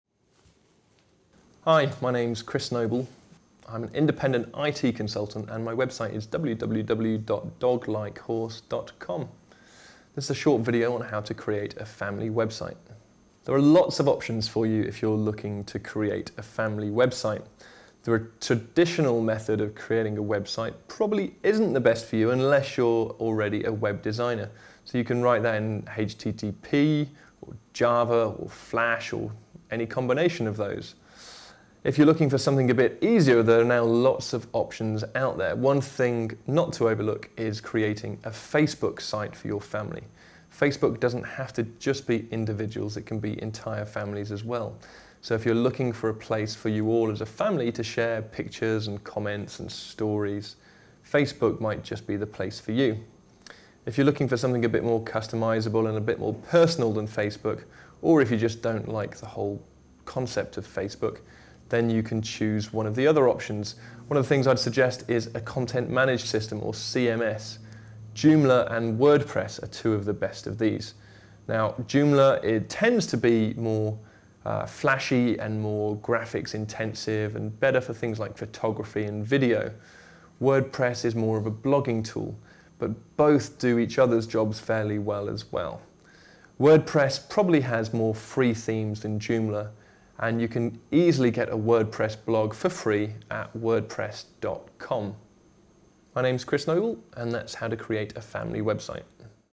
Computer Basics : Create a Family Website
Creating a family website can be done the traditional way using programming languages, or you can create a family Facebook page for example. Learn the different options for creating a family website from an IT consultant in this free video on websites.

Bio: Christopher Noble is an IT entrepreneur who was instrumental in building two successful IT companies.